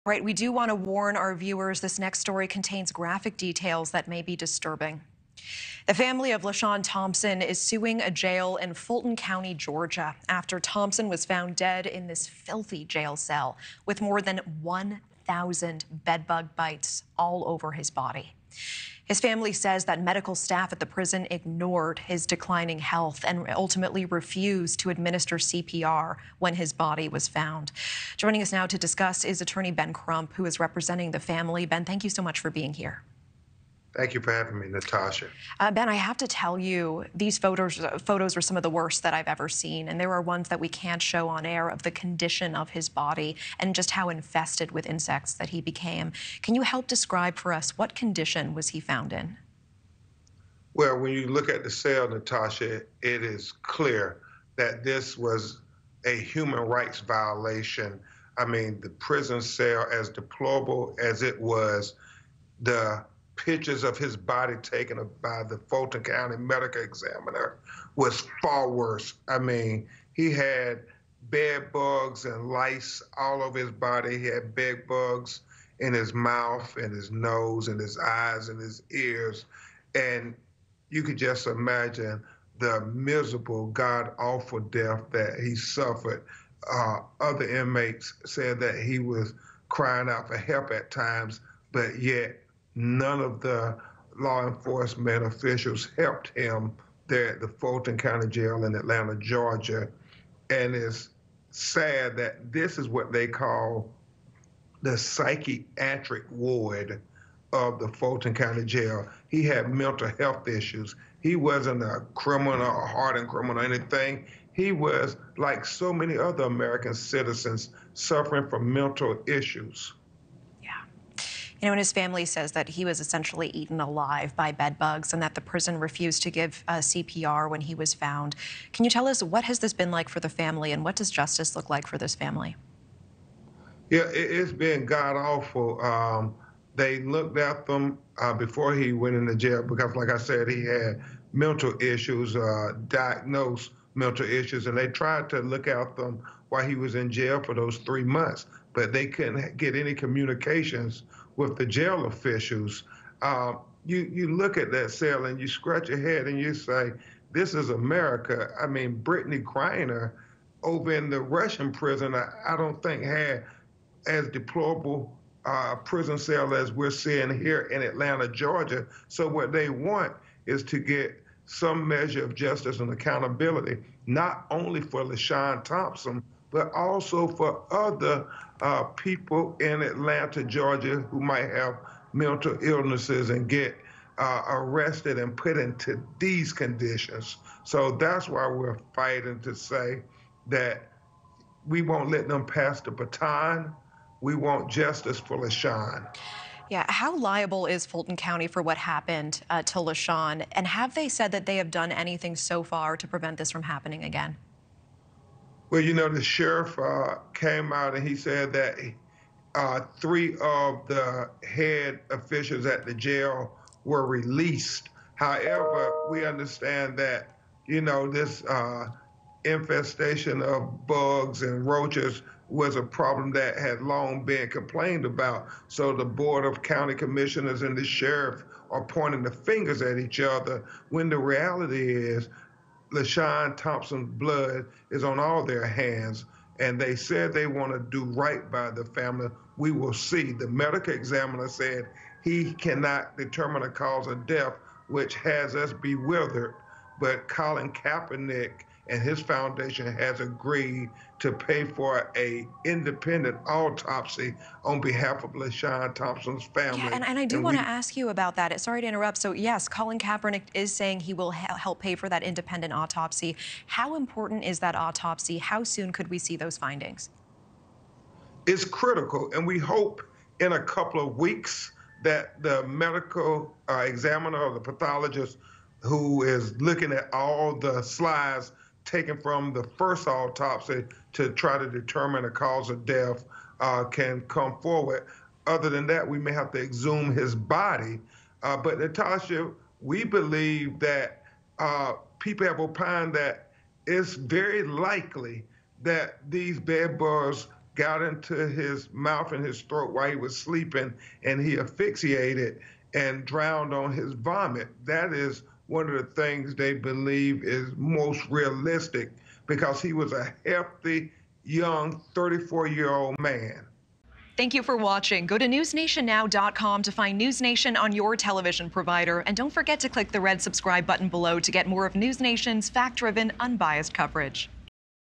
‘We want justice’: Family sues jail after inmate allegedly ‘eaten alive by bed bugs’ | NewsNation
Warning: This video contains graphic details and images some may find disturbing. The family of Lashawn Thompson is seeking justice after the 34-year-old man was allegedly found dead in a bed bug infested jail cell in Georgia. Attorney Ben Crump calls Thompson’s death a “human rights violation.”